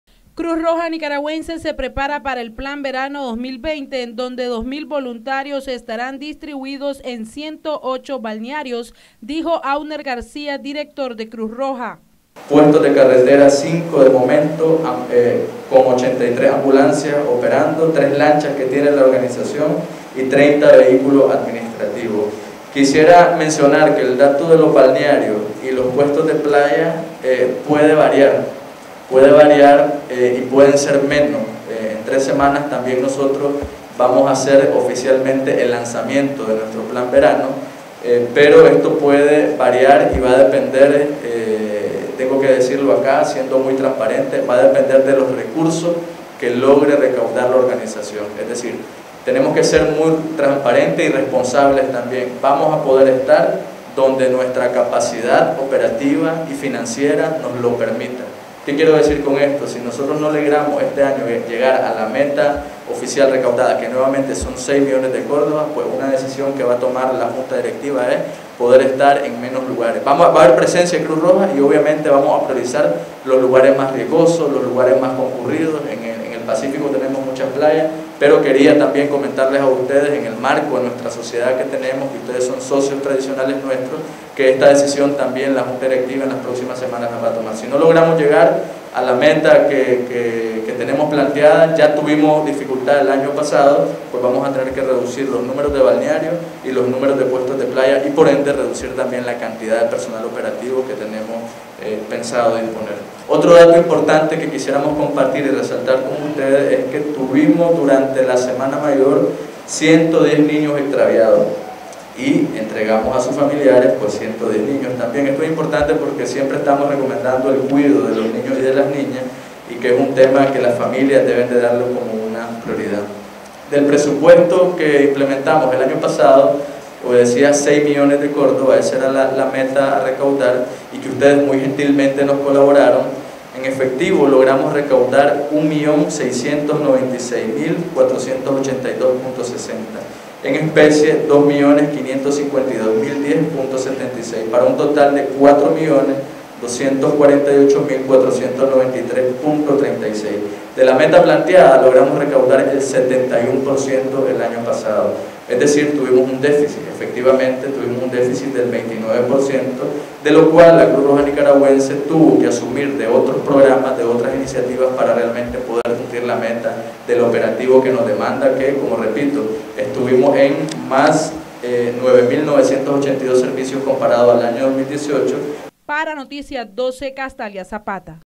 Cruz Roja Nicaragüense se prepara para el plan verano 2020
Miembros de la Cruz Roja Nicaragüense se preparan para dar inicio al plan verano 2020.